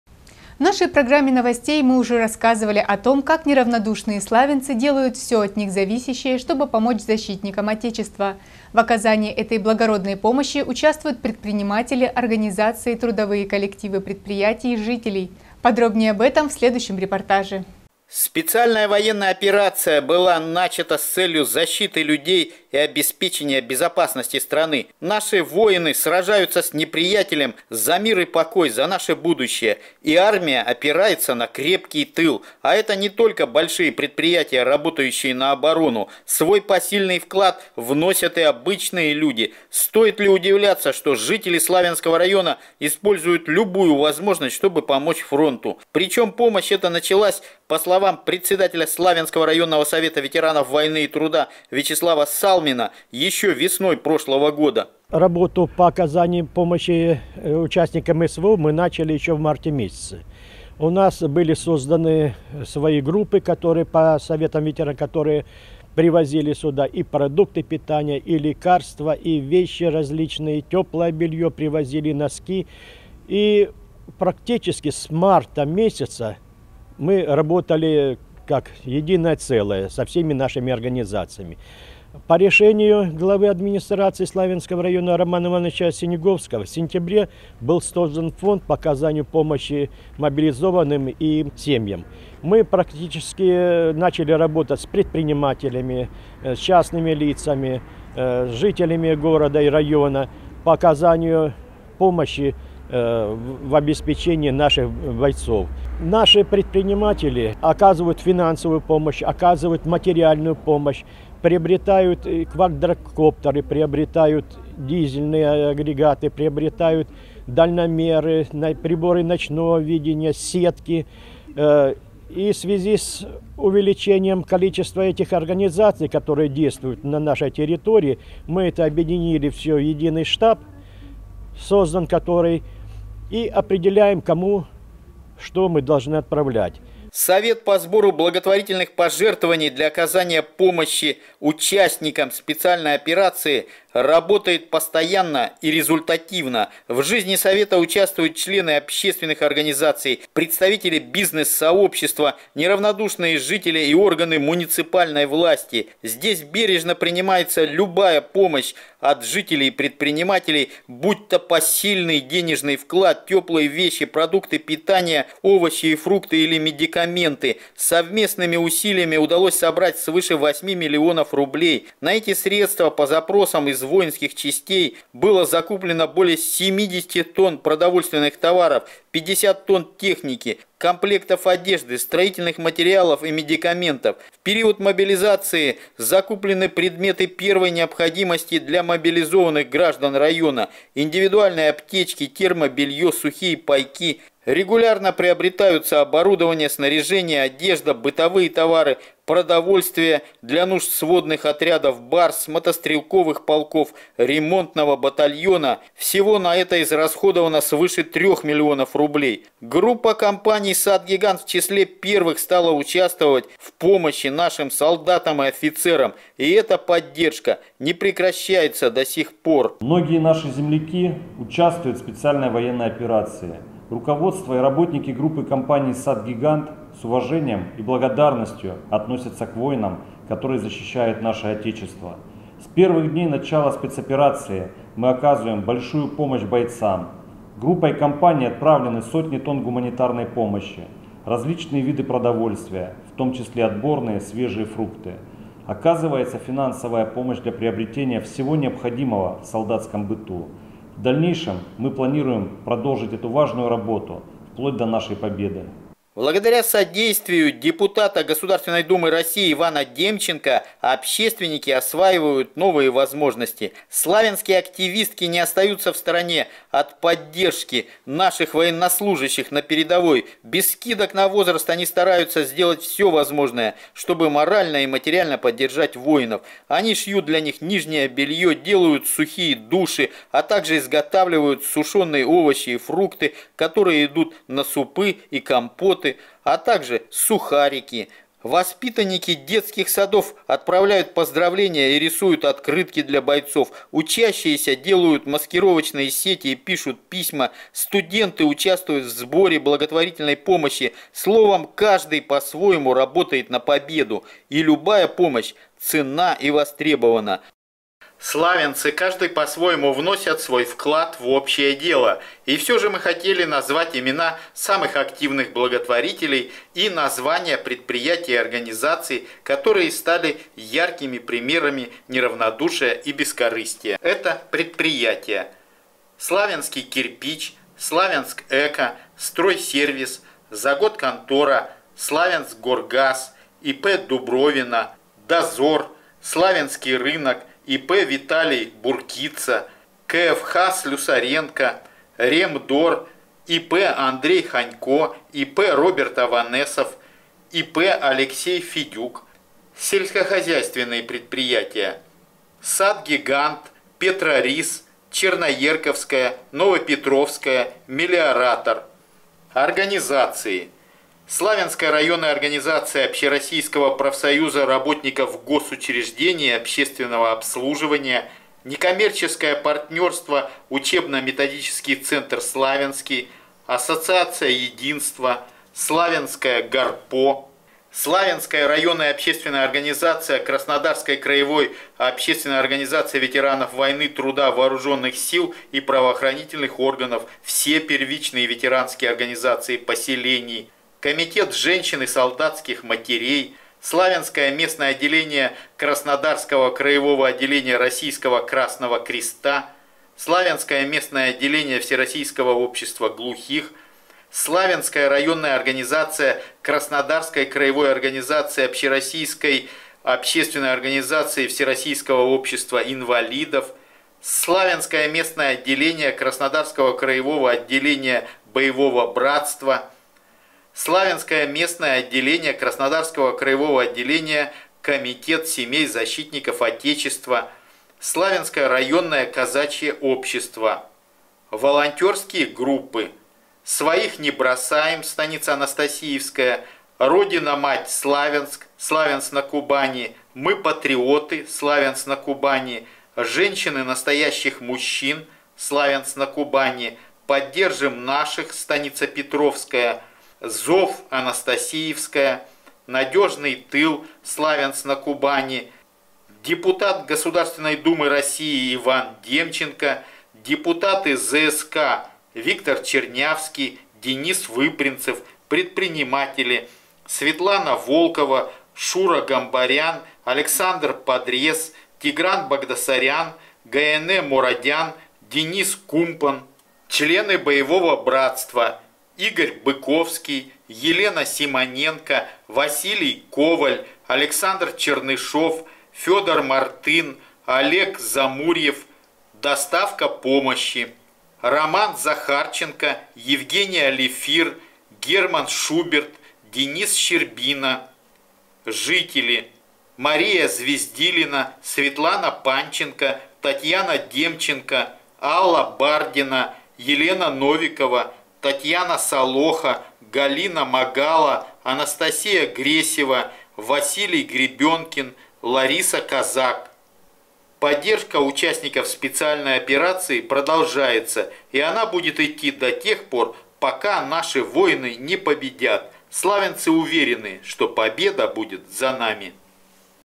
Реальная помощь фронту
Неравнодушные славянцы делают все от них зависящее, чтобы помочь защитникам Отечества. В оказании этой благородной помощи участвуют предприниматели, организации, трудовые коллективы предприятий, жители.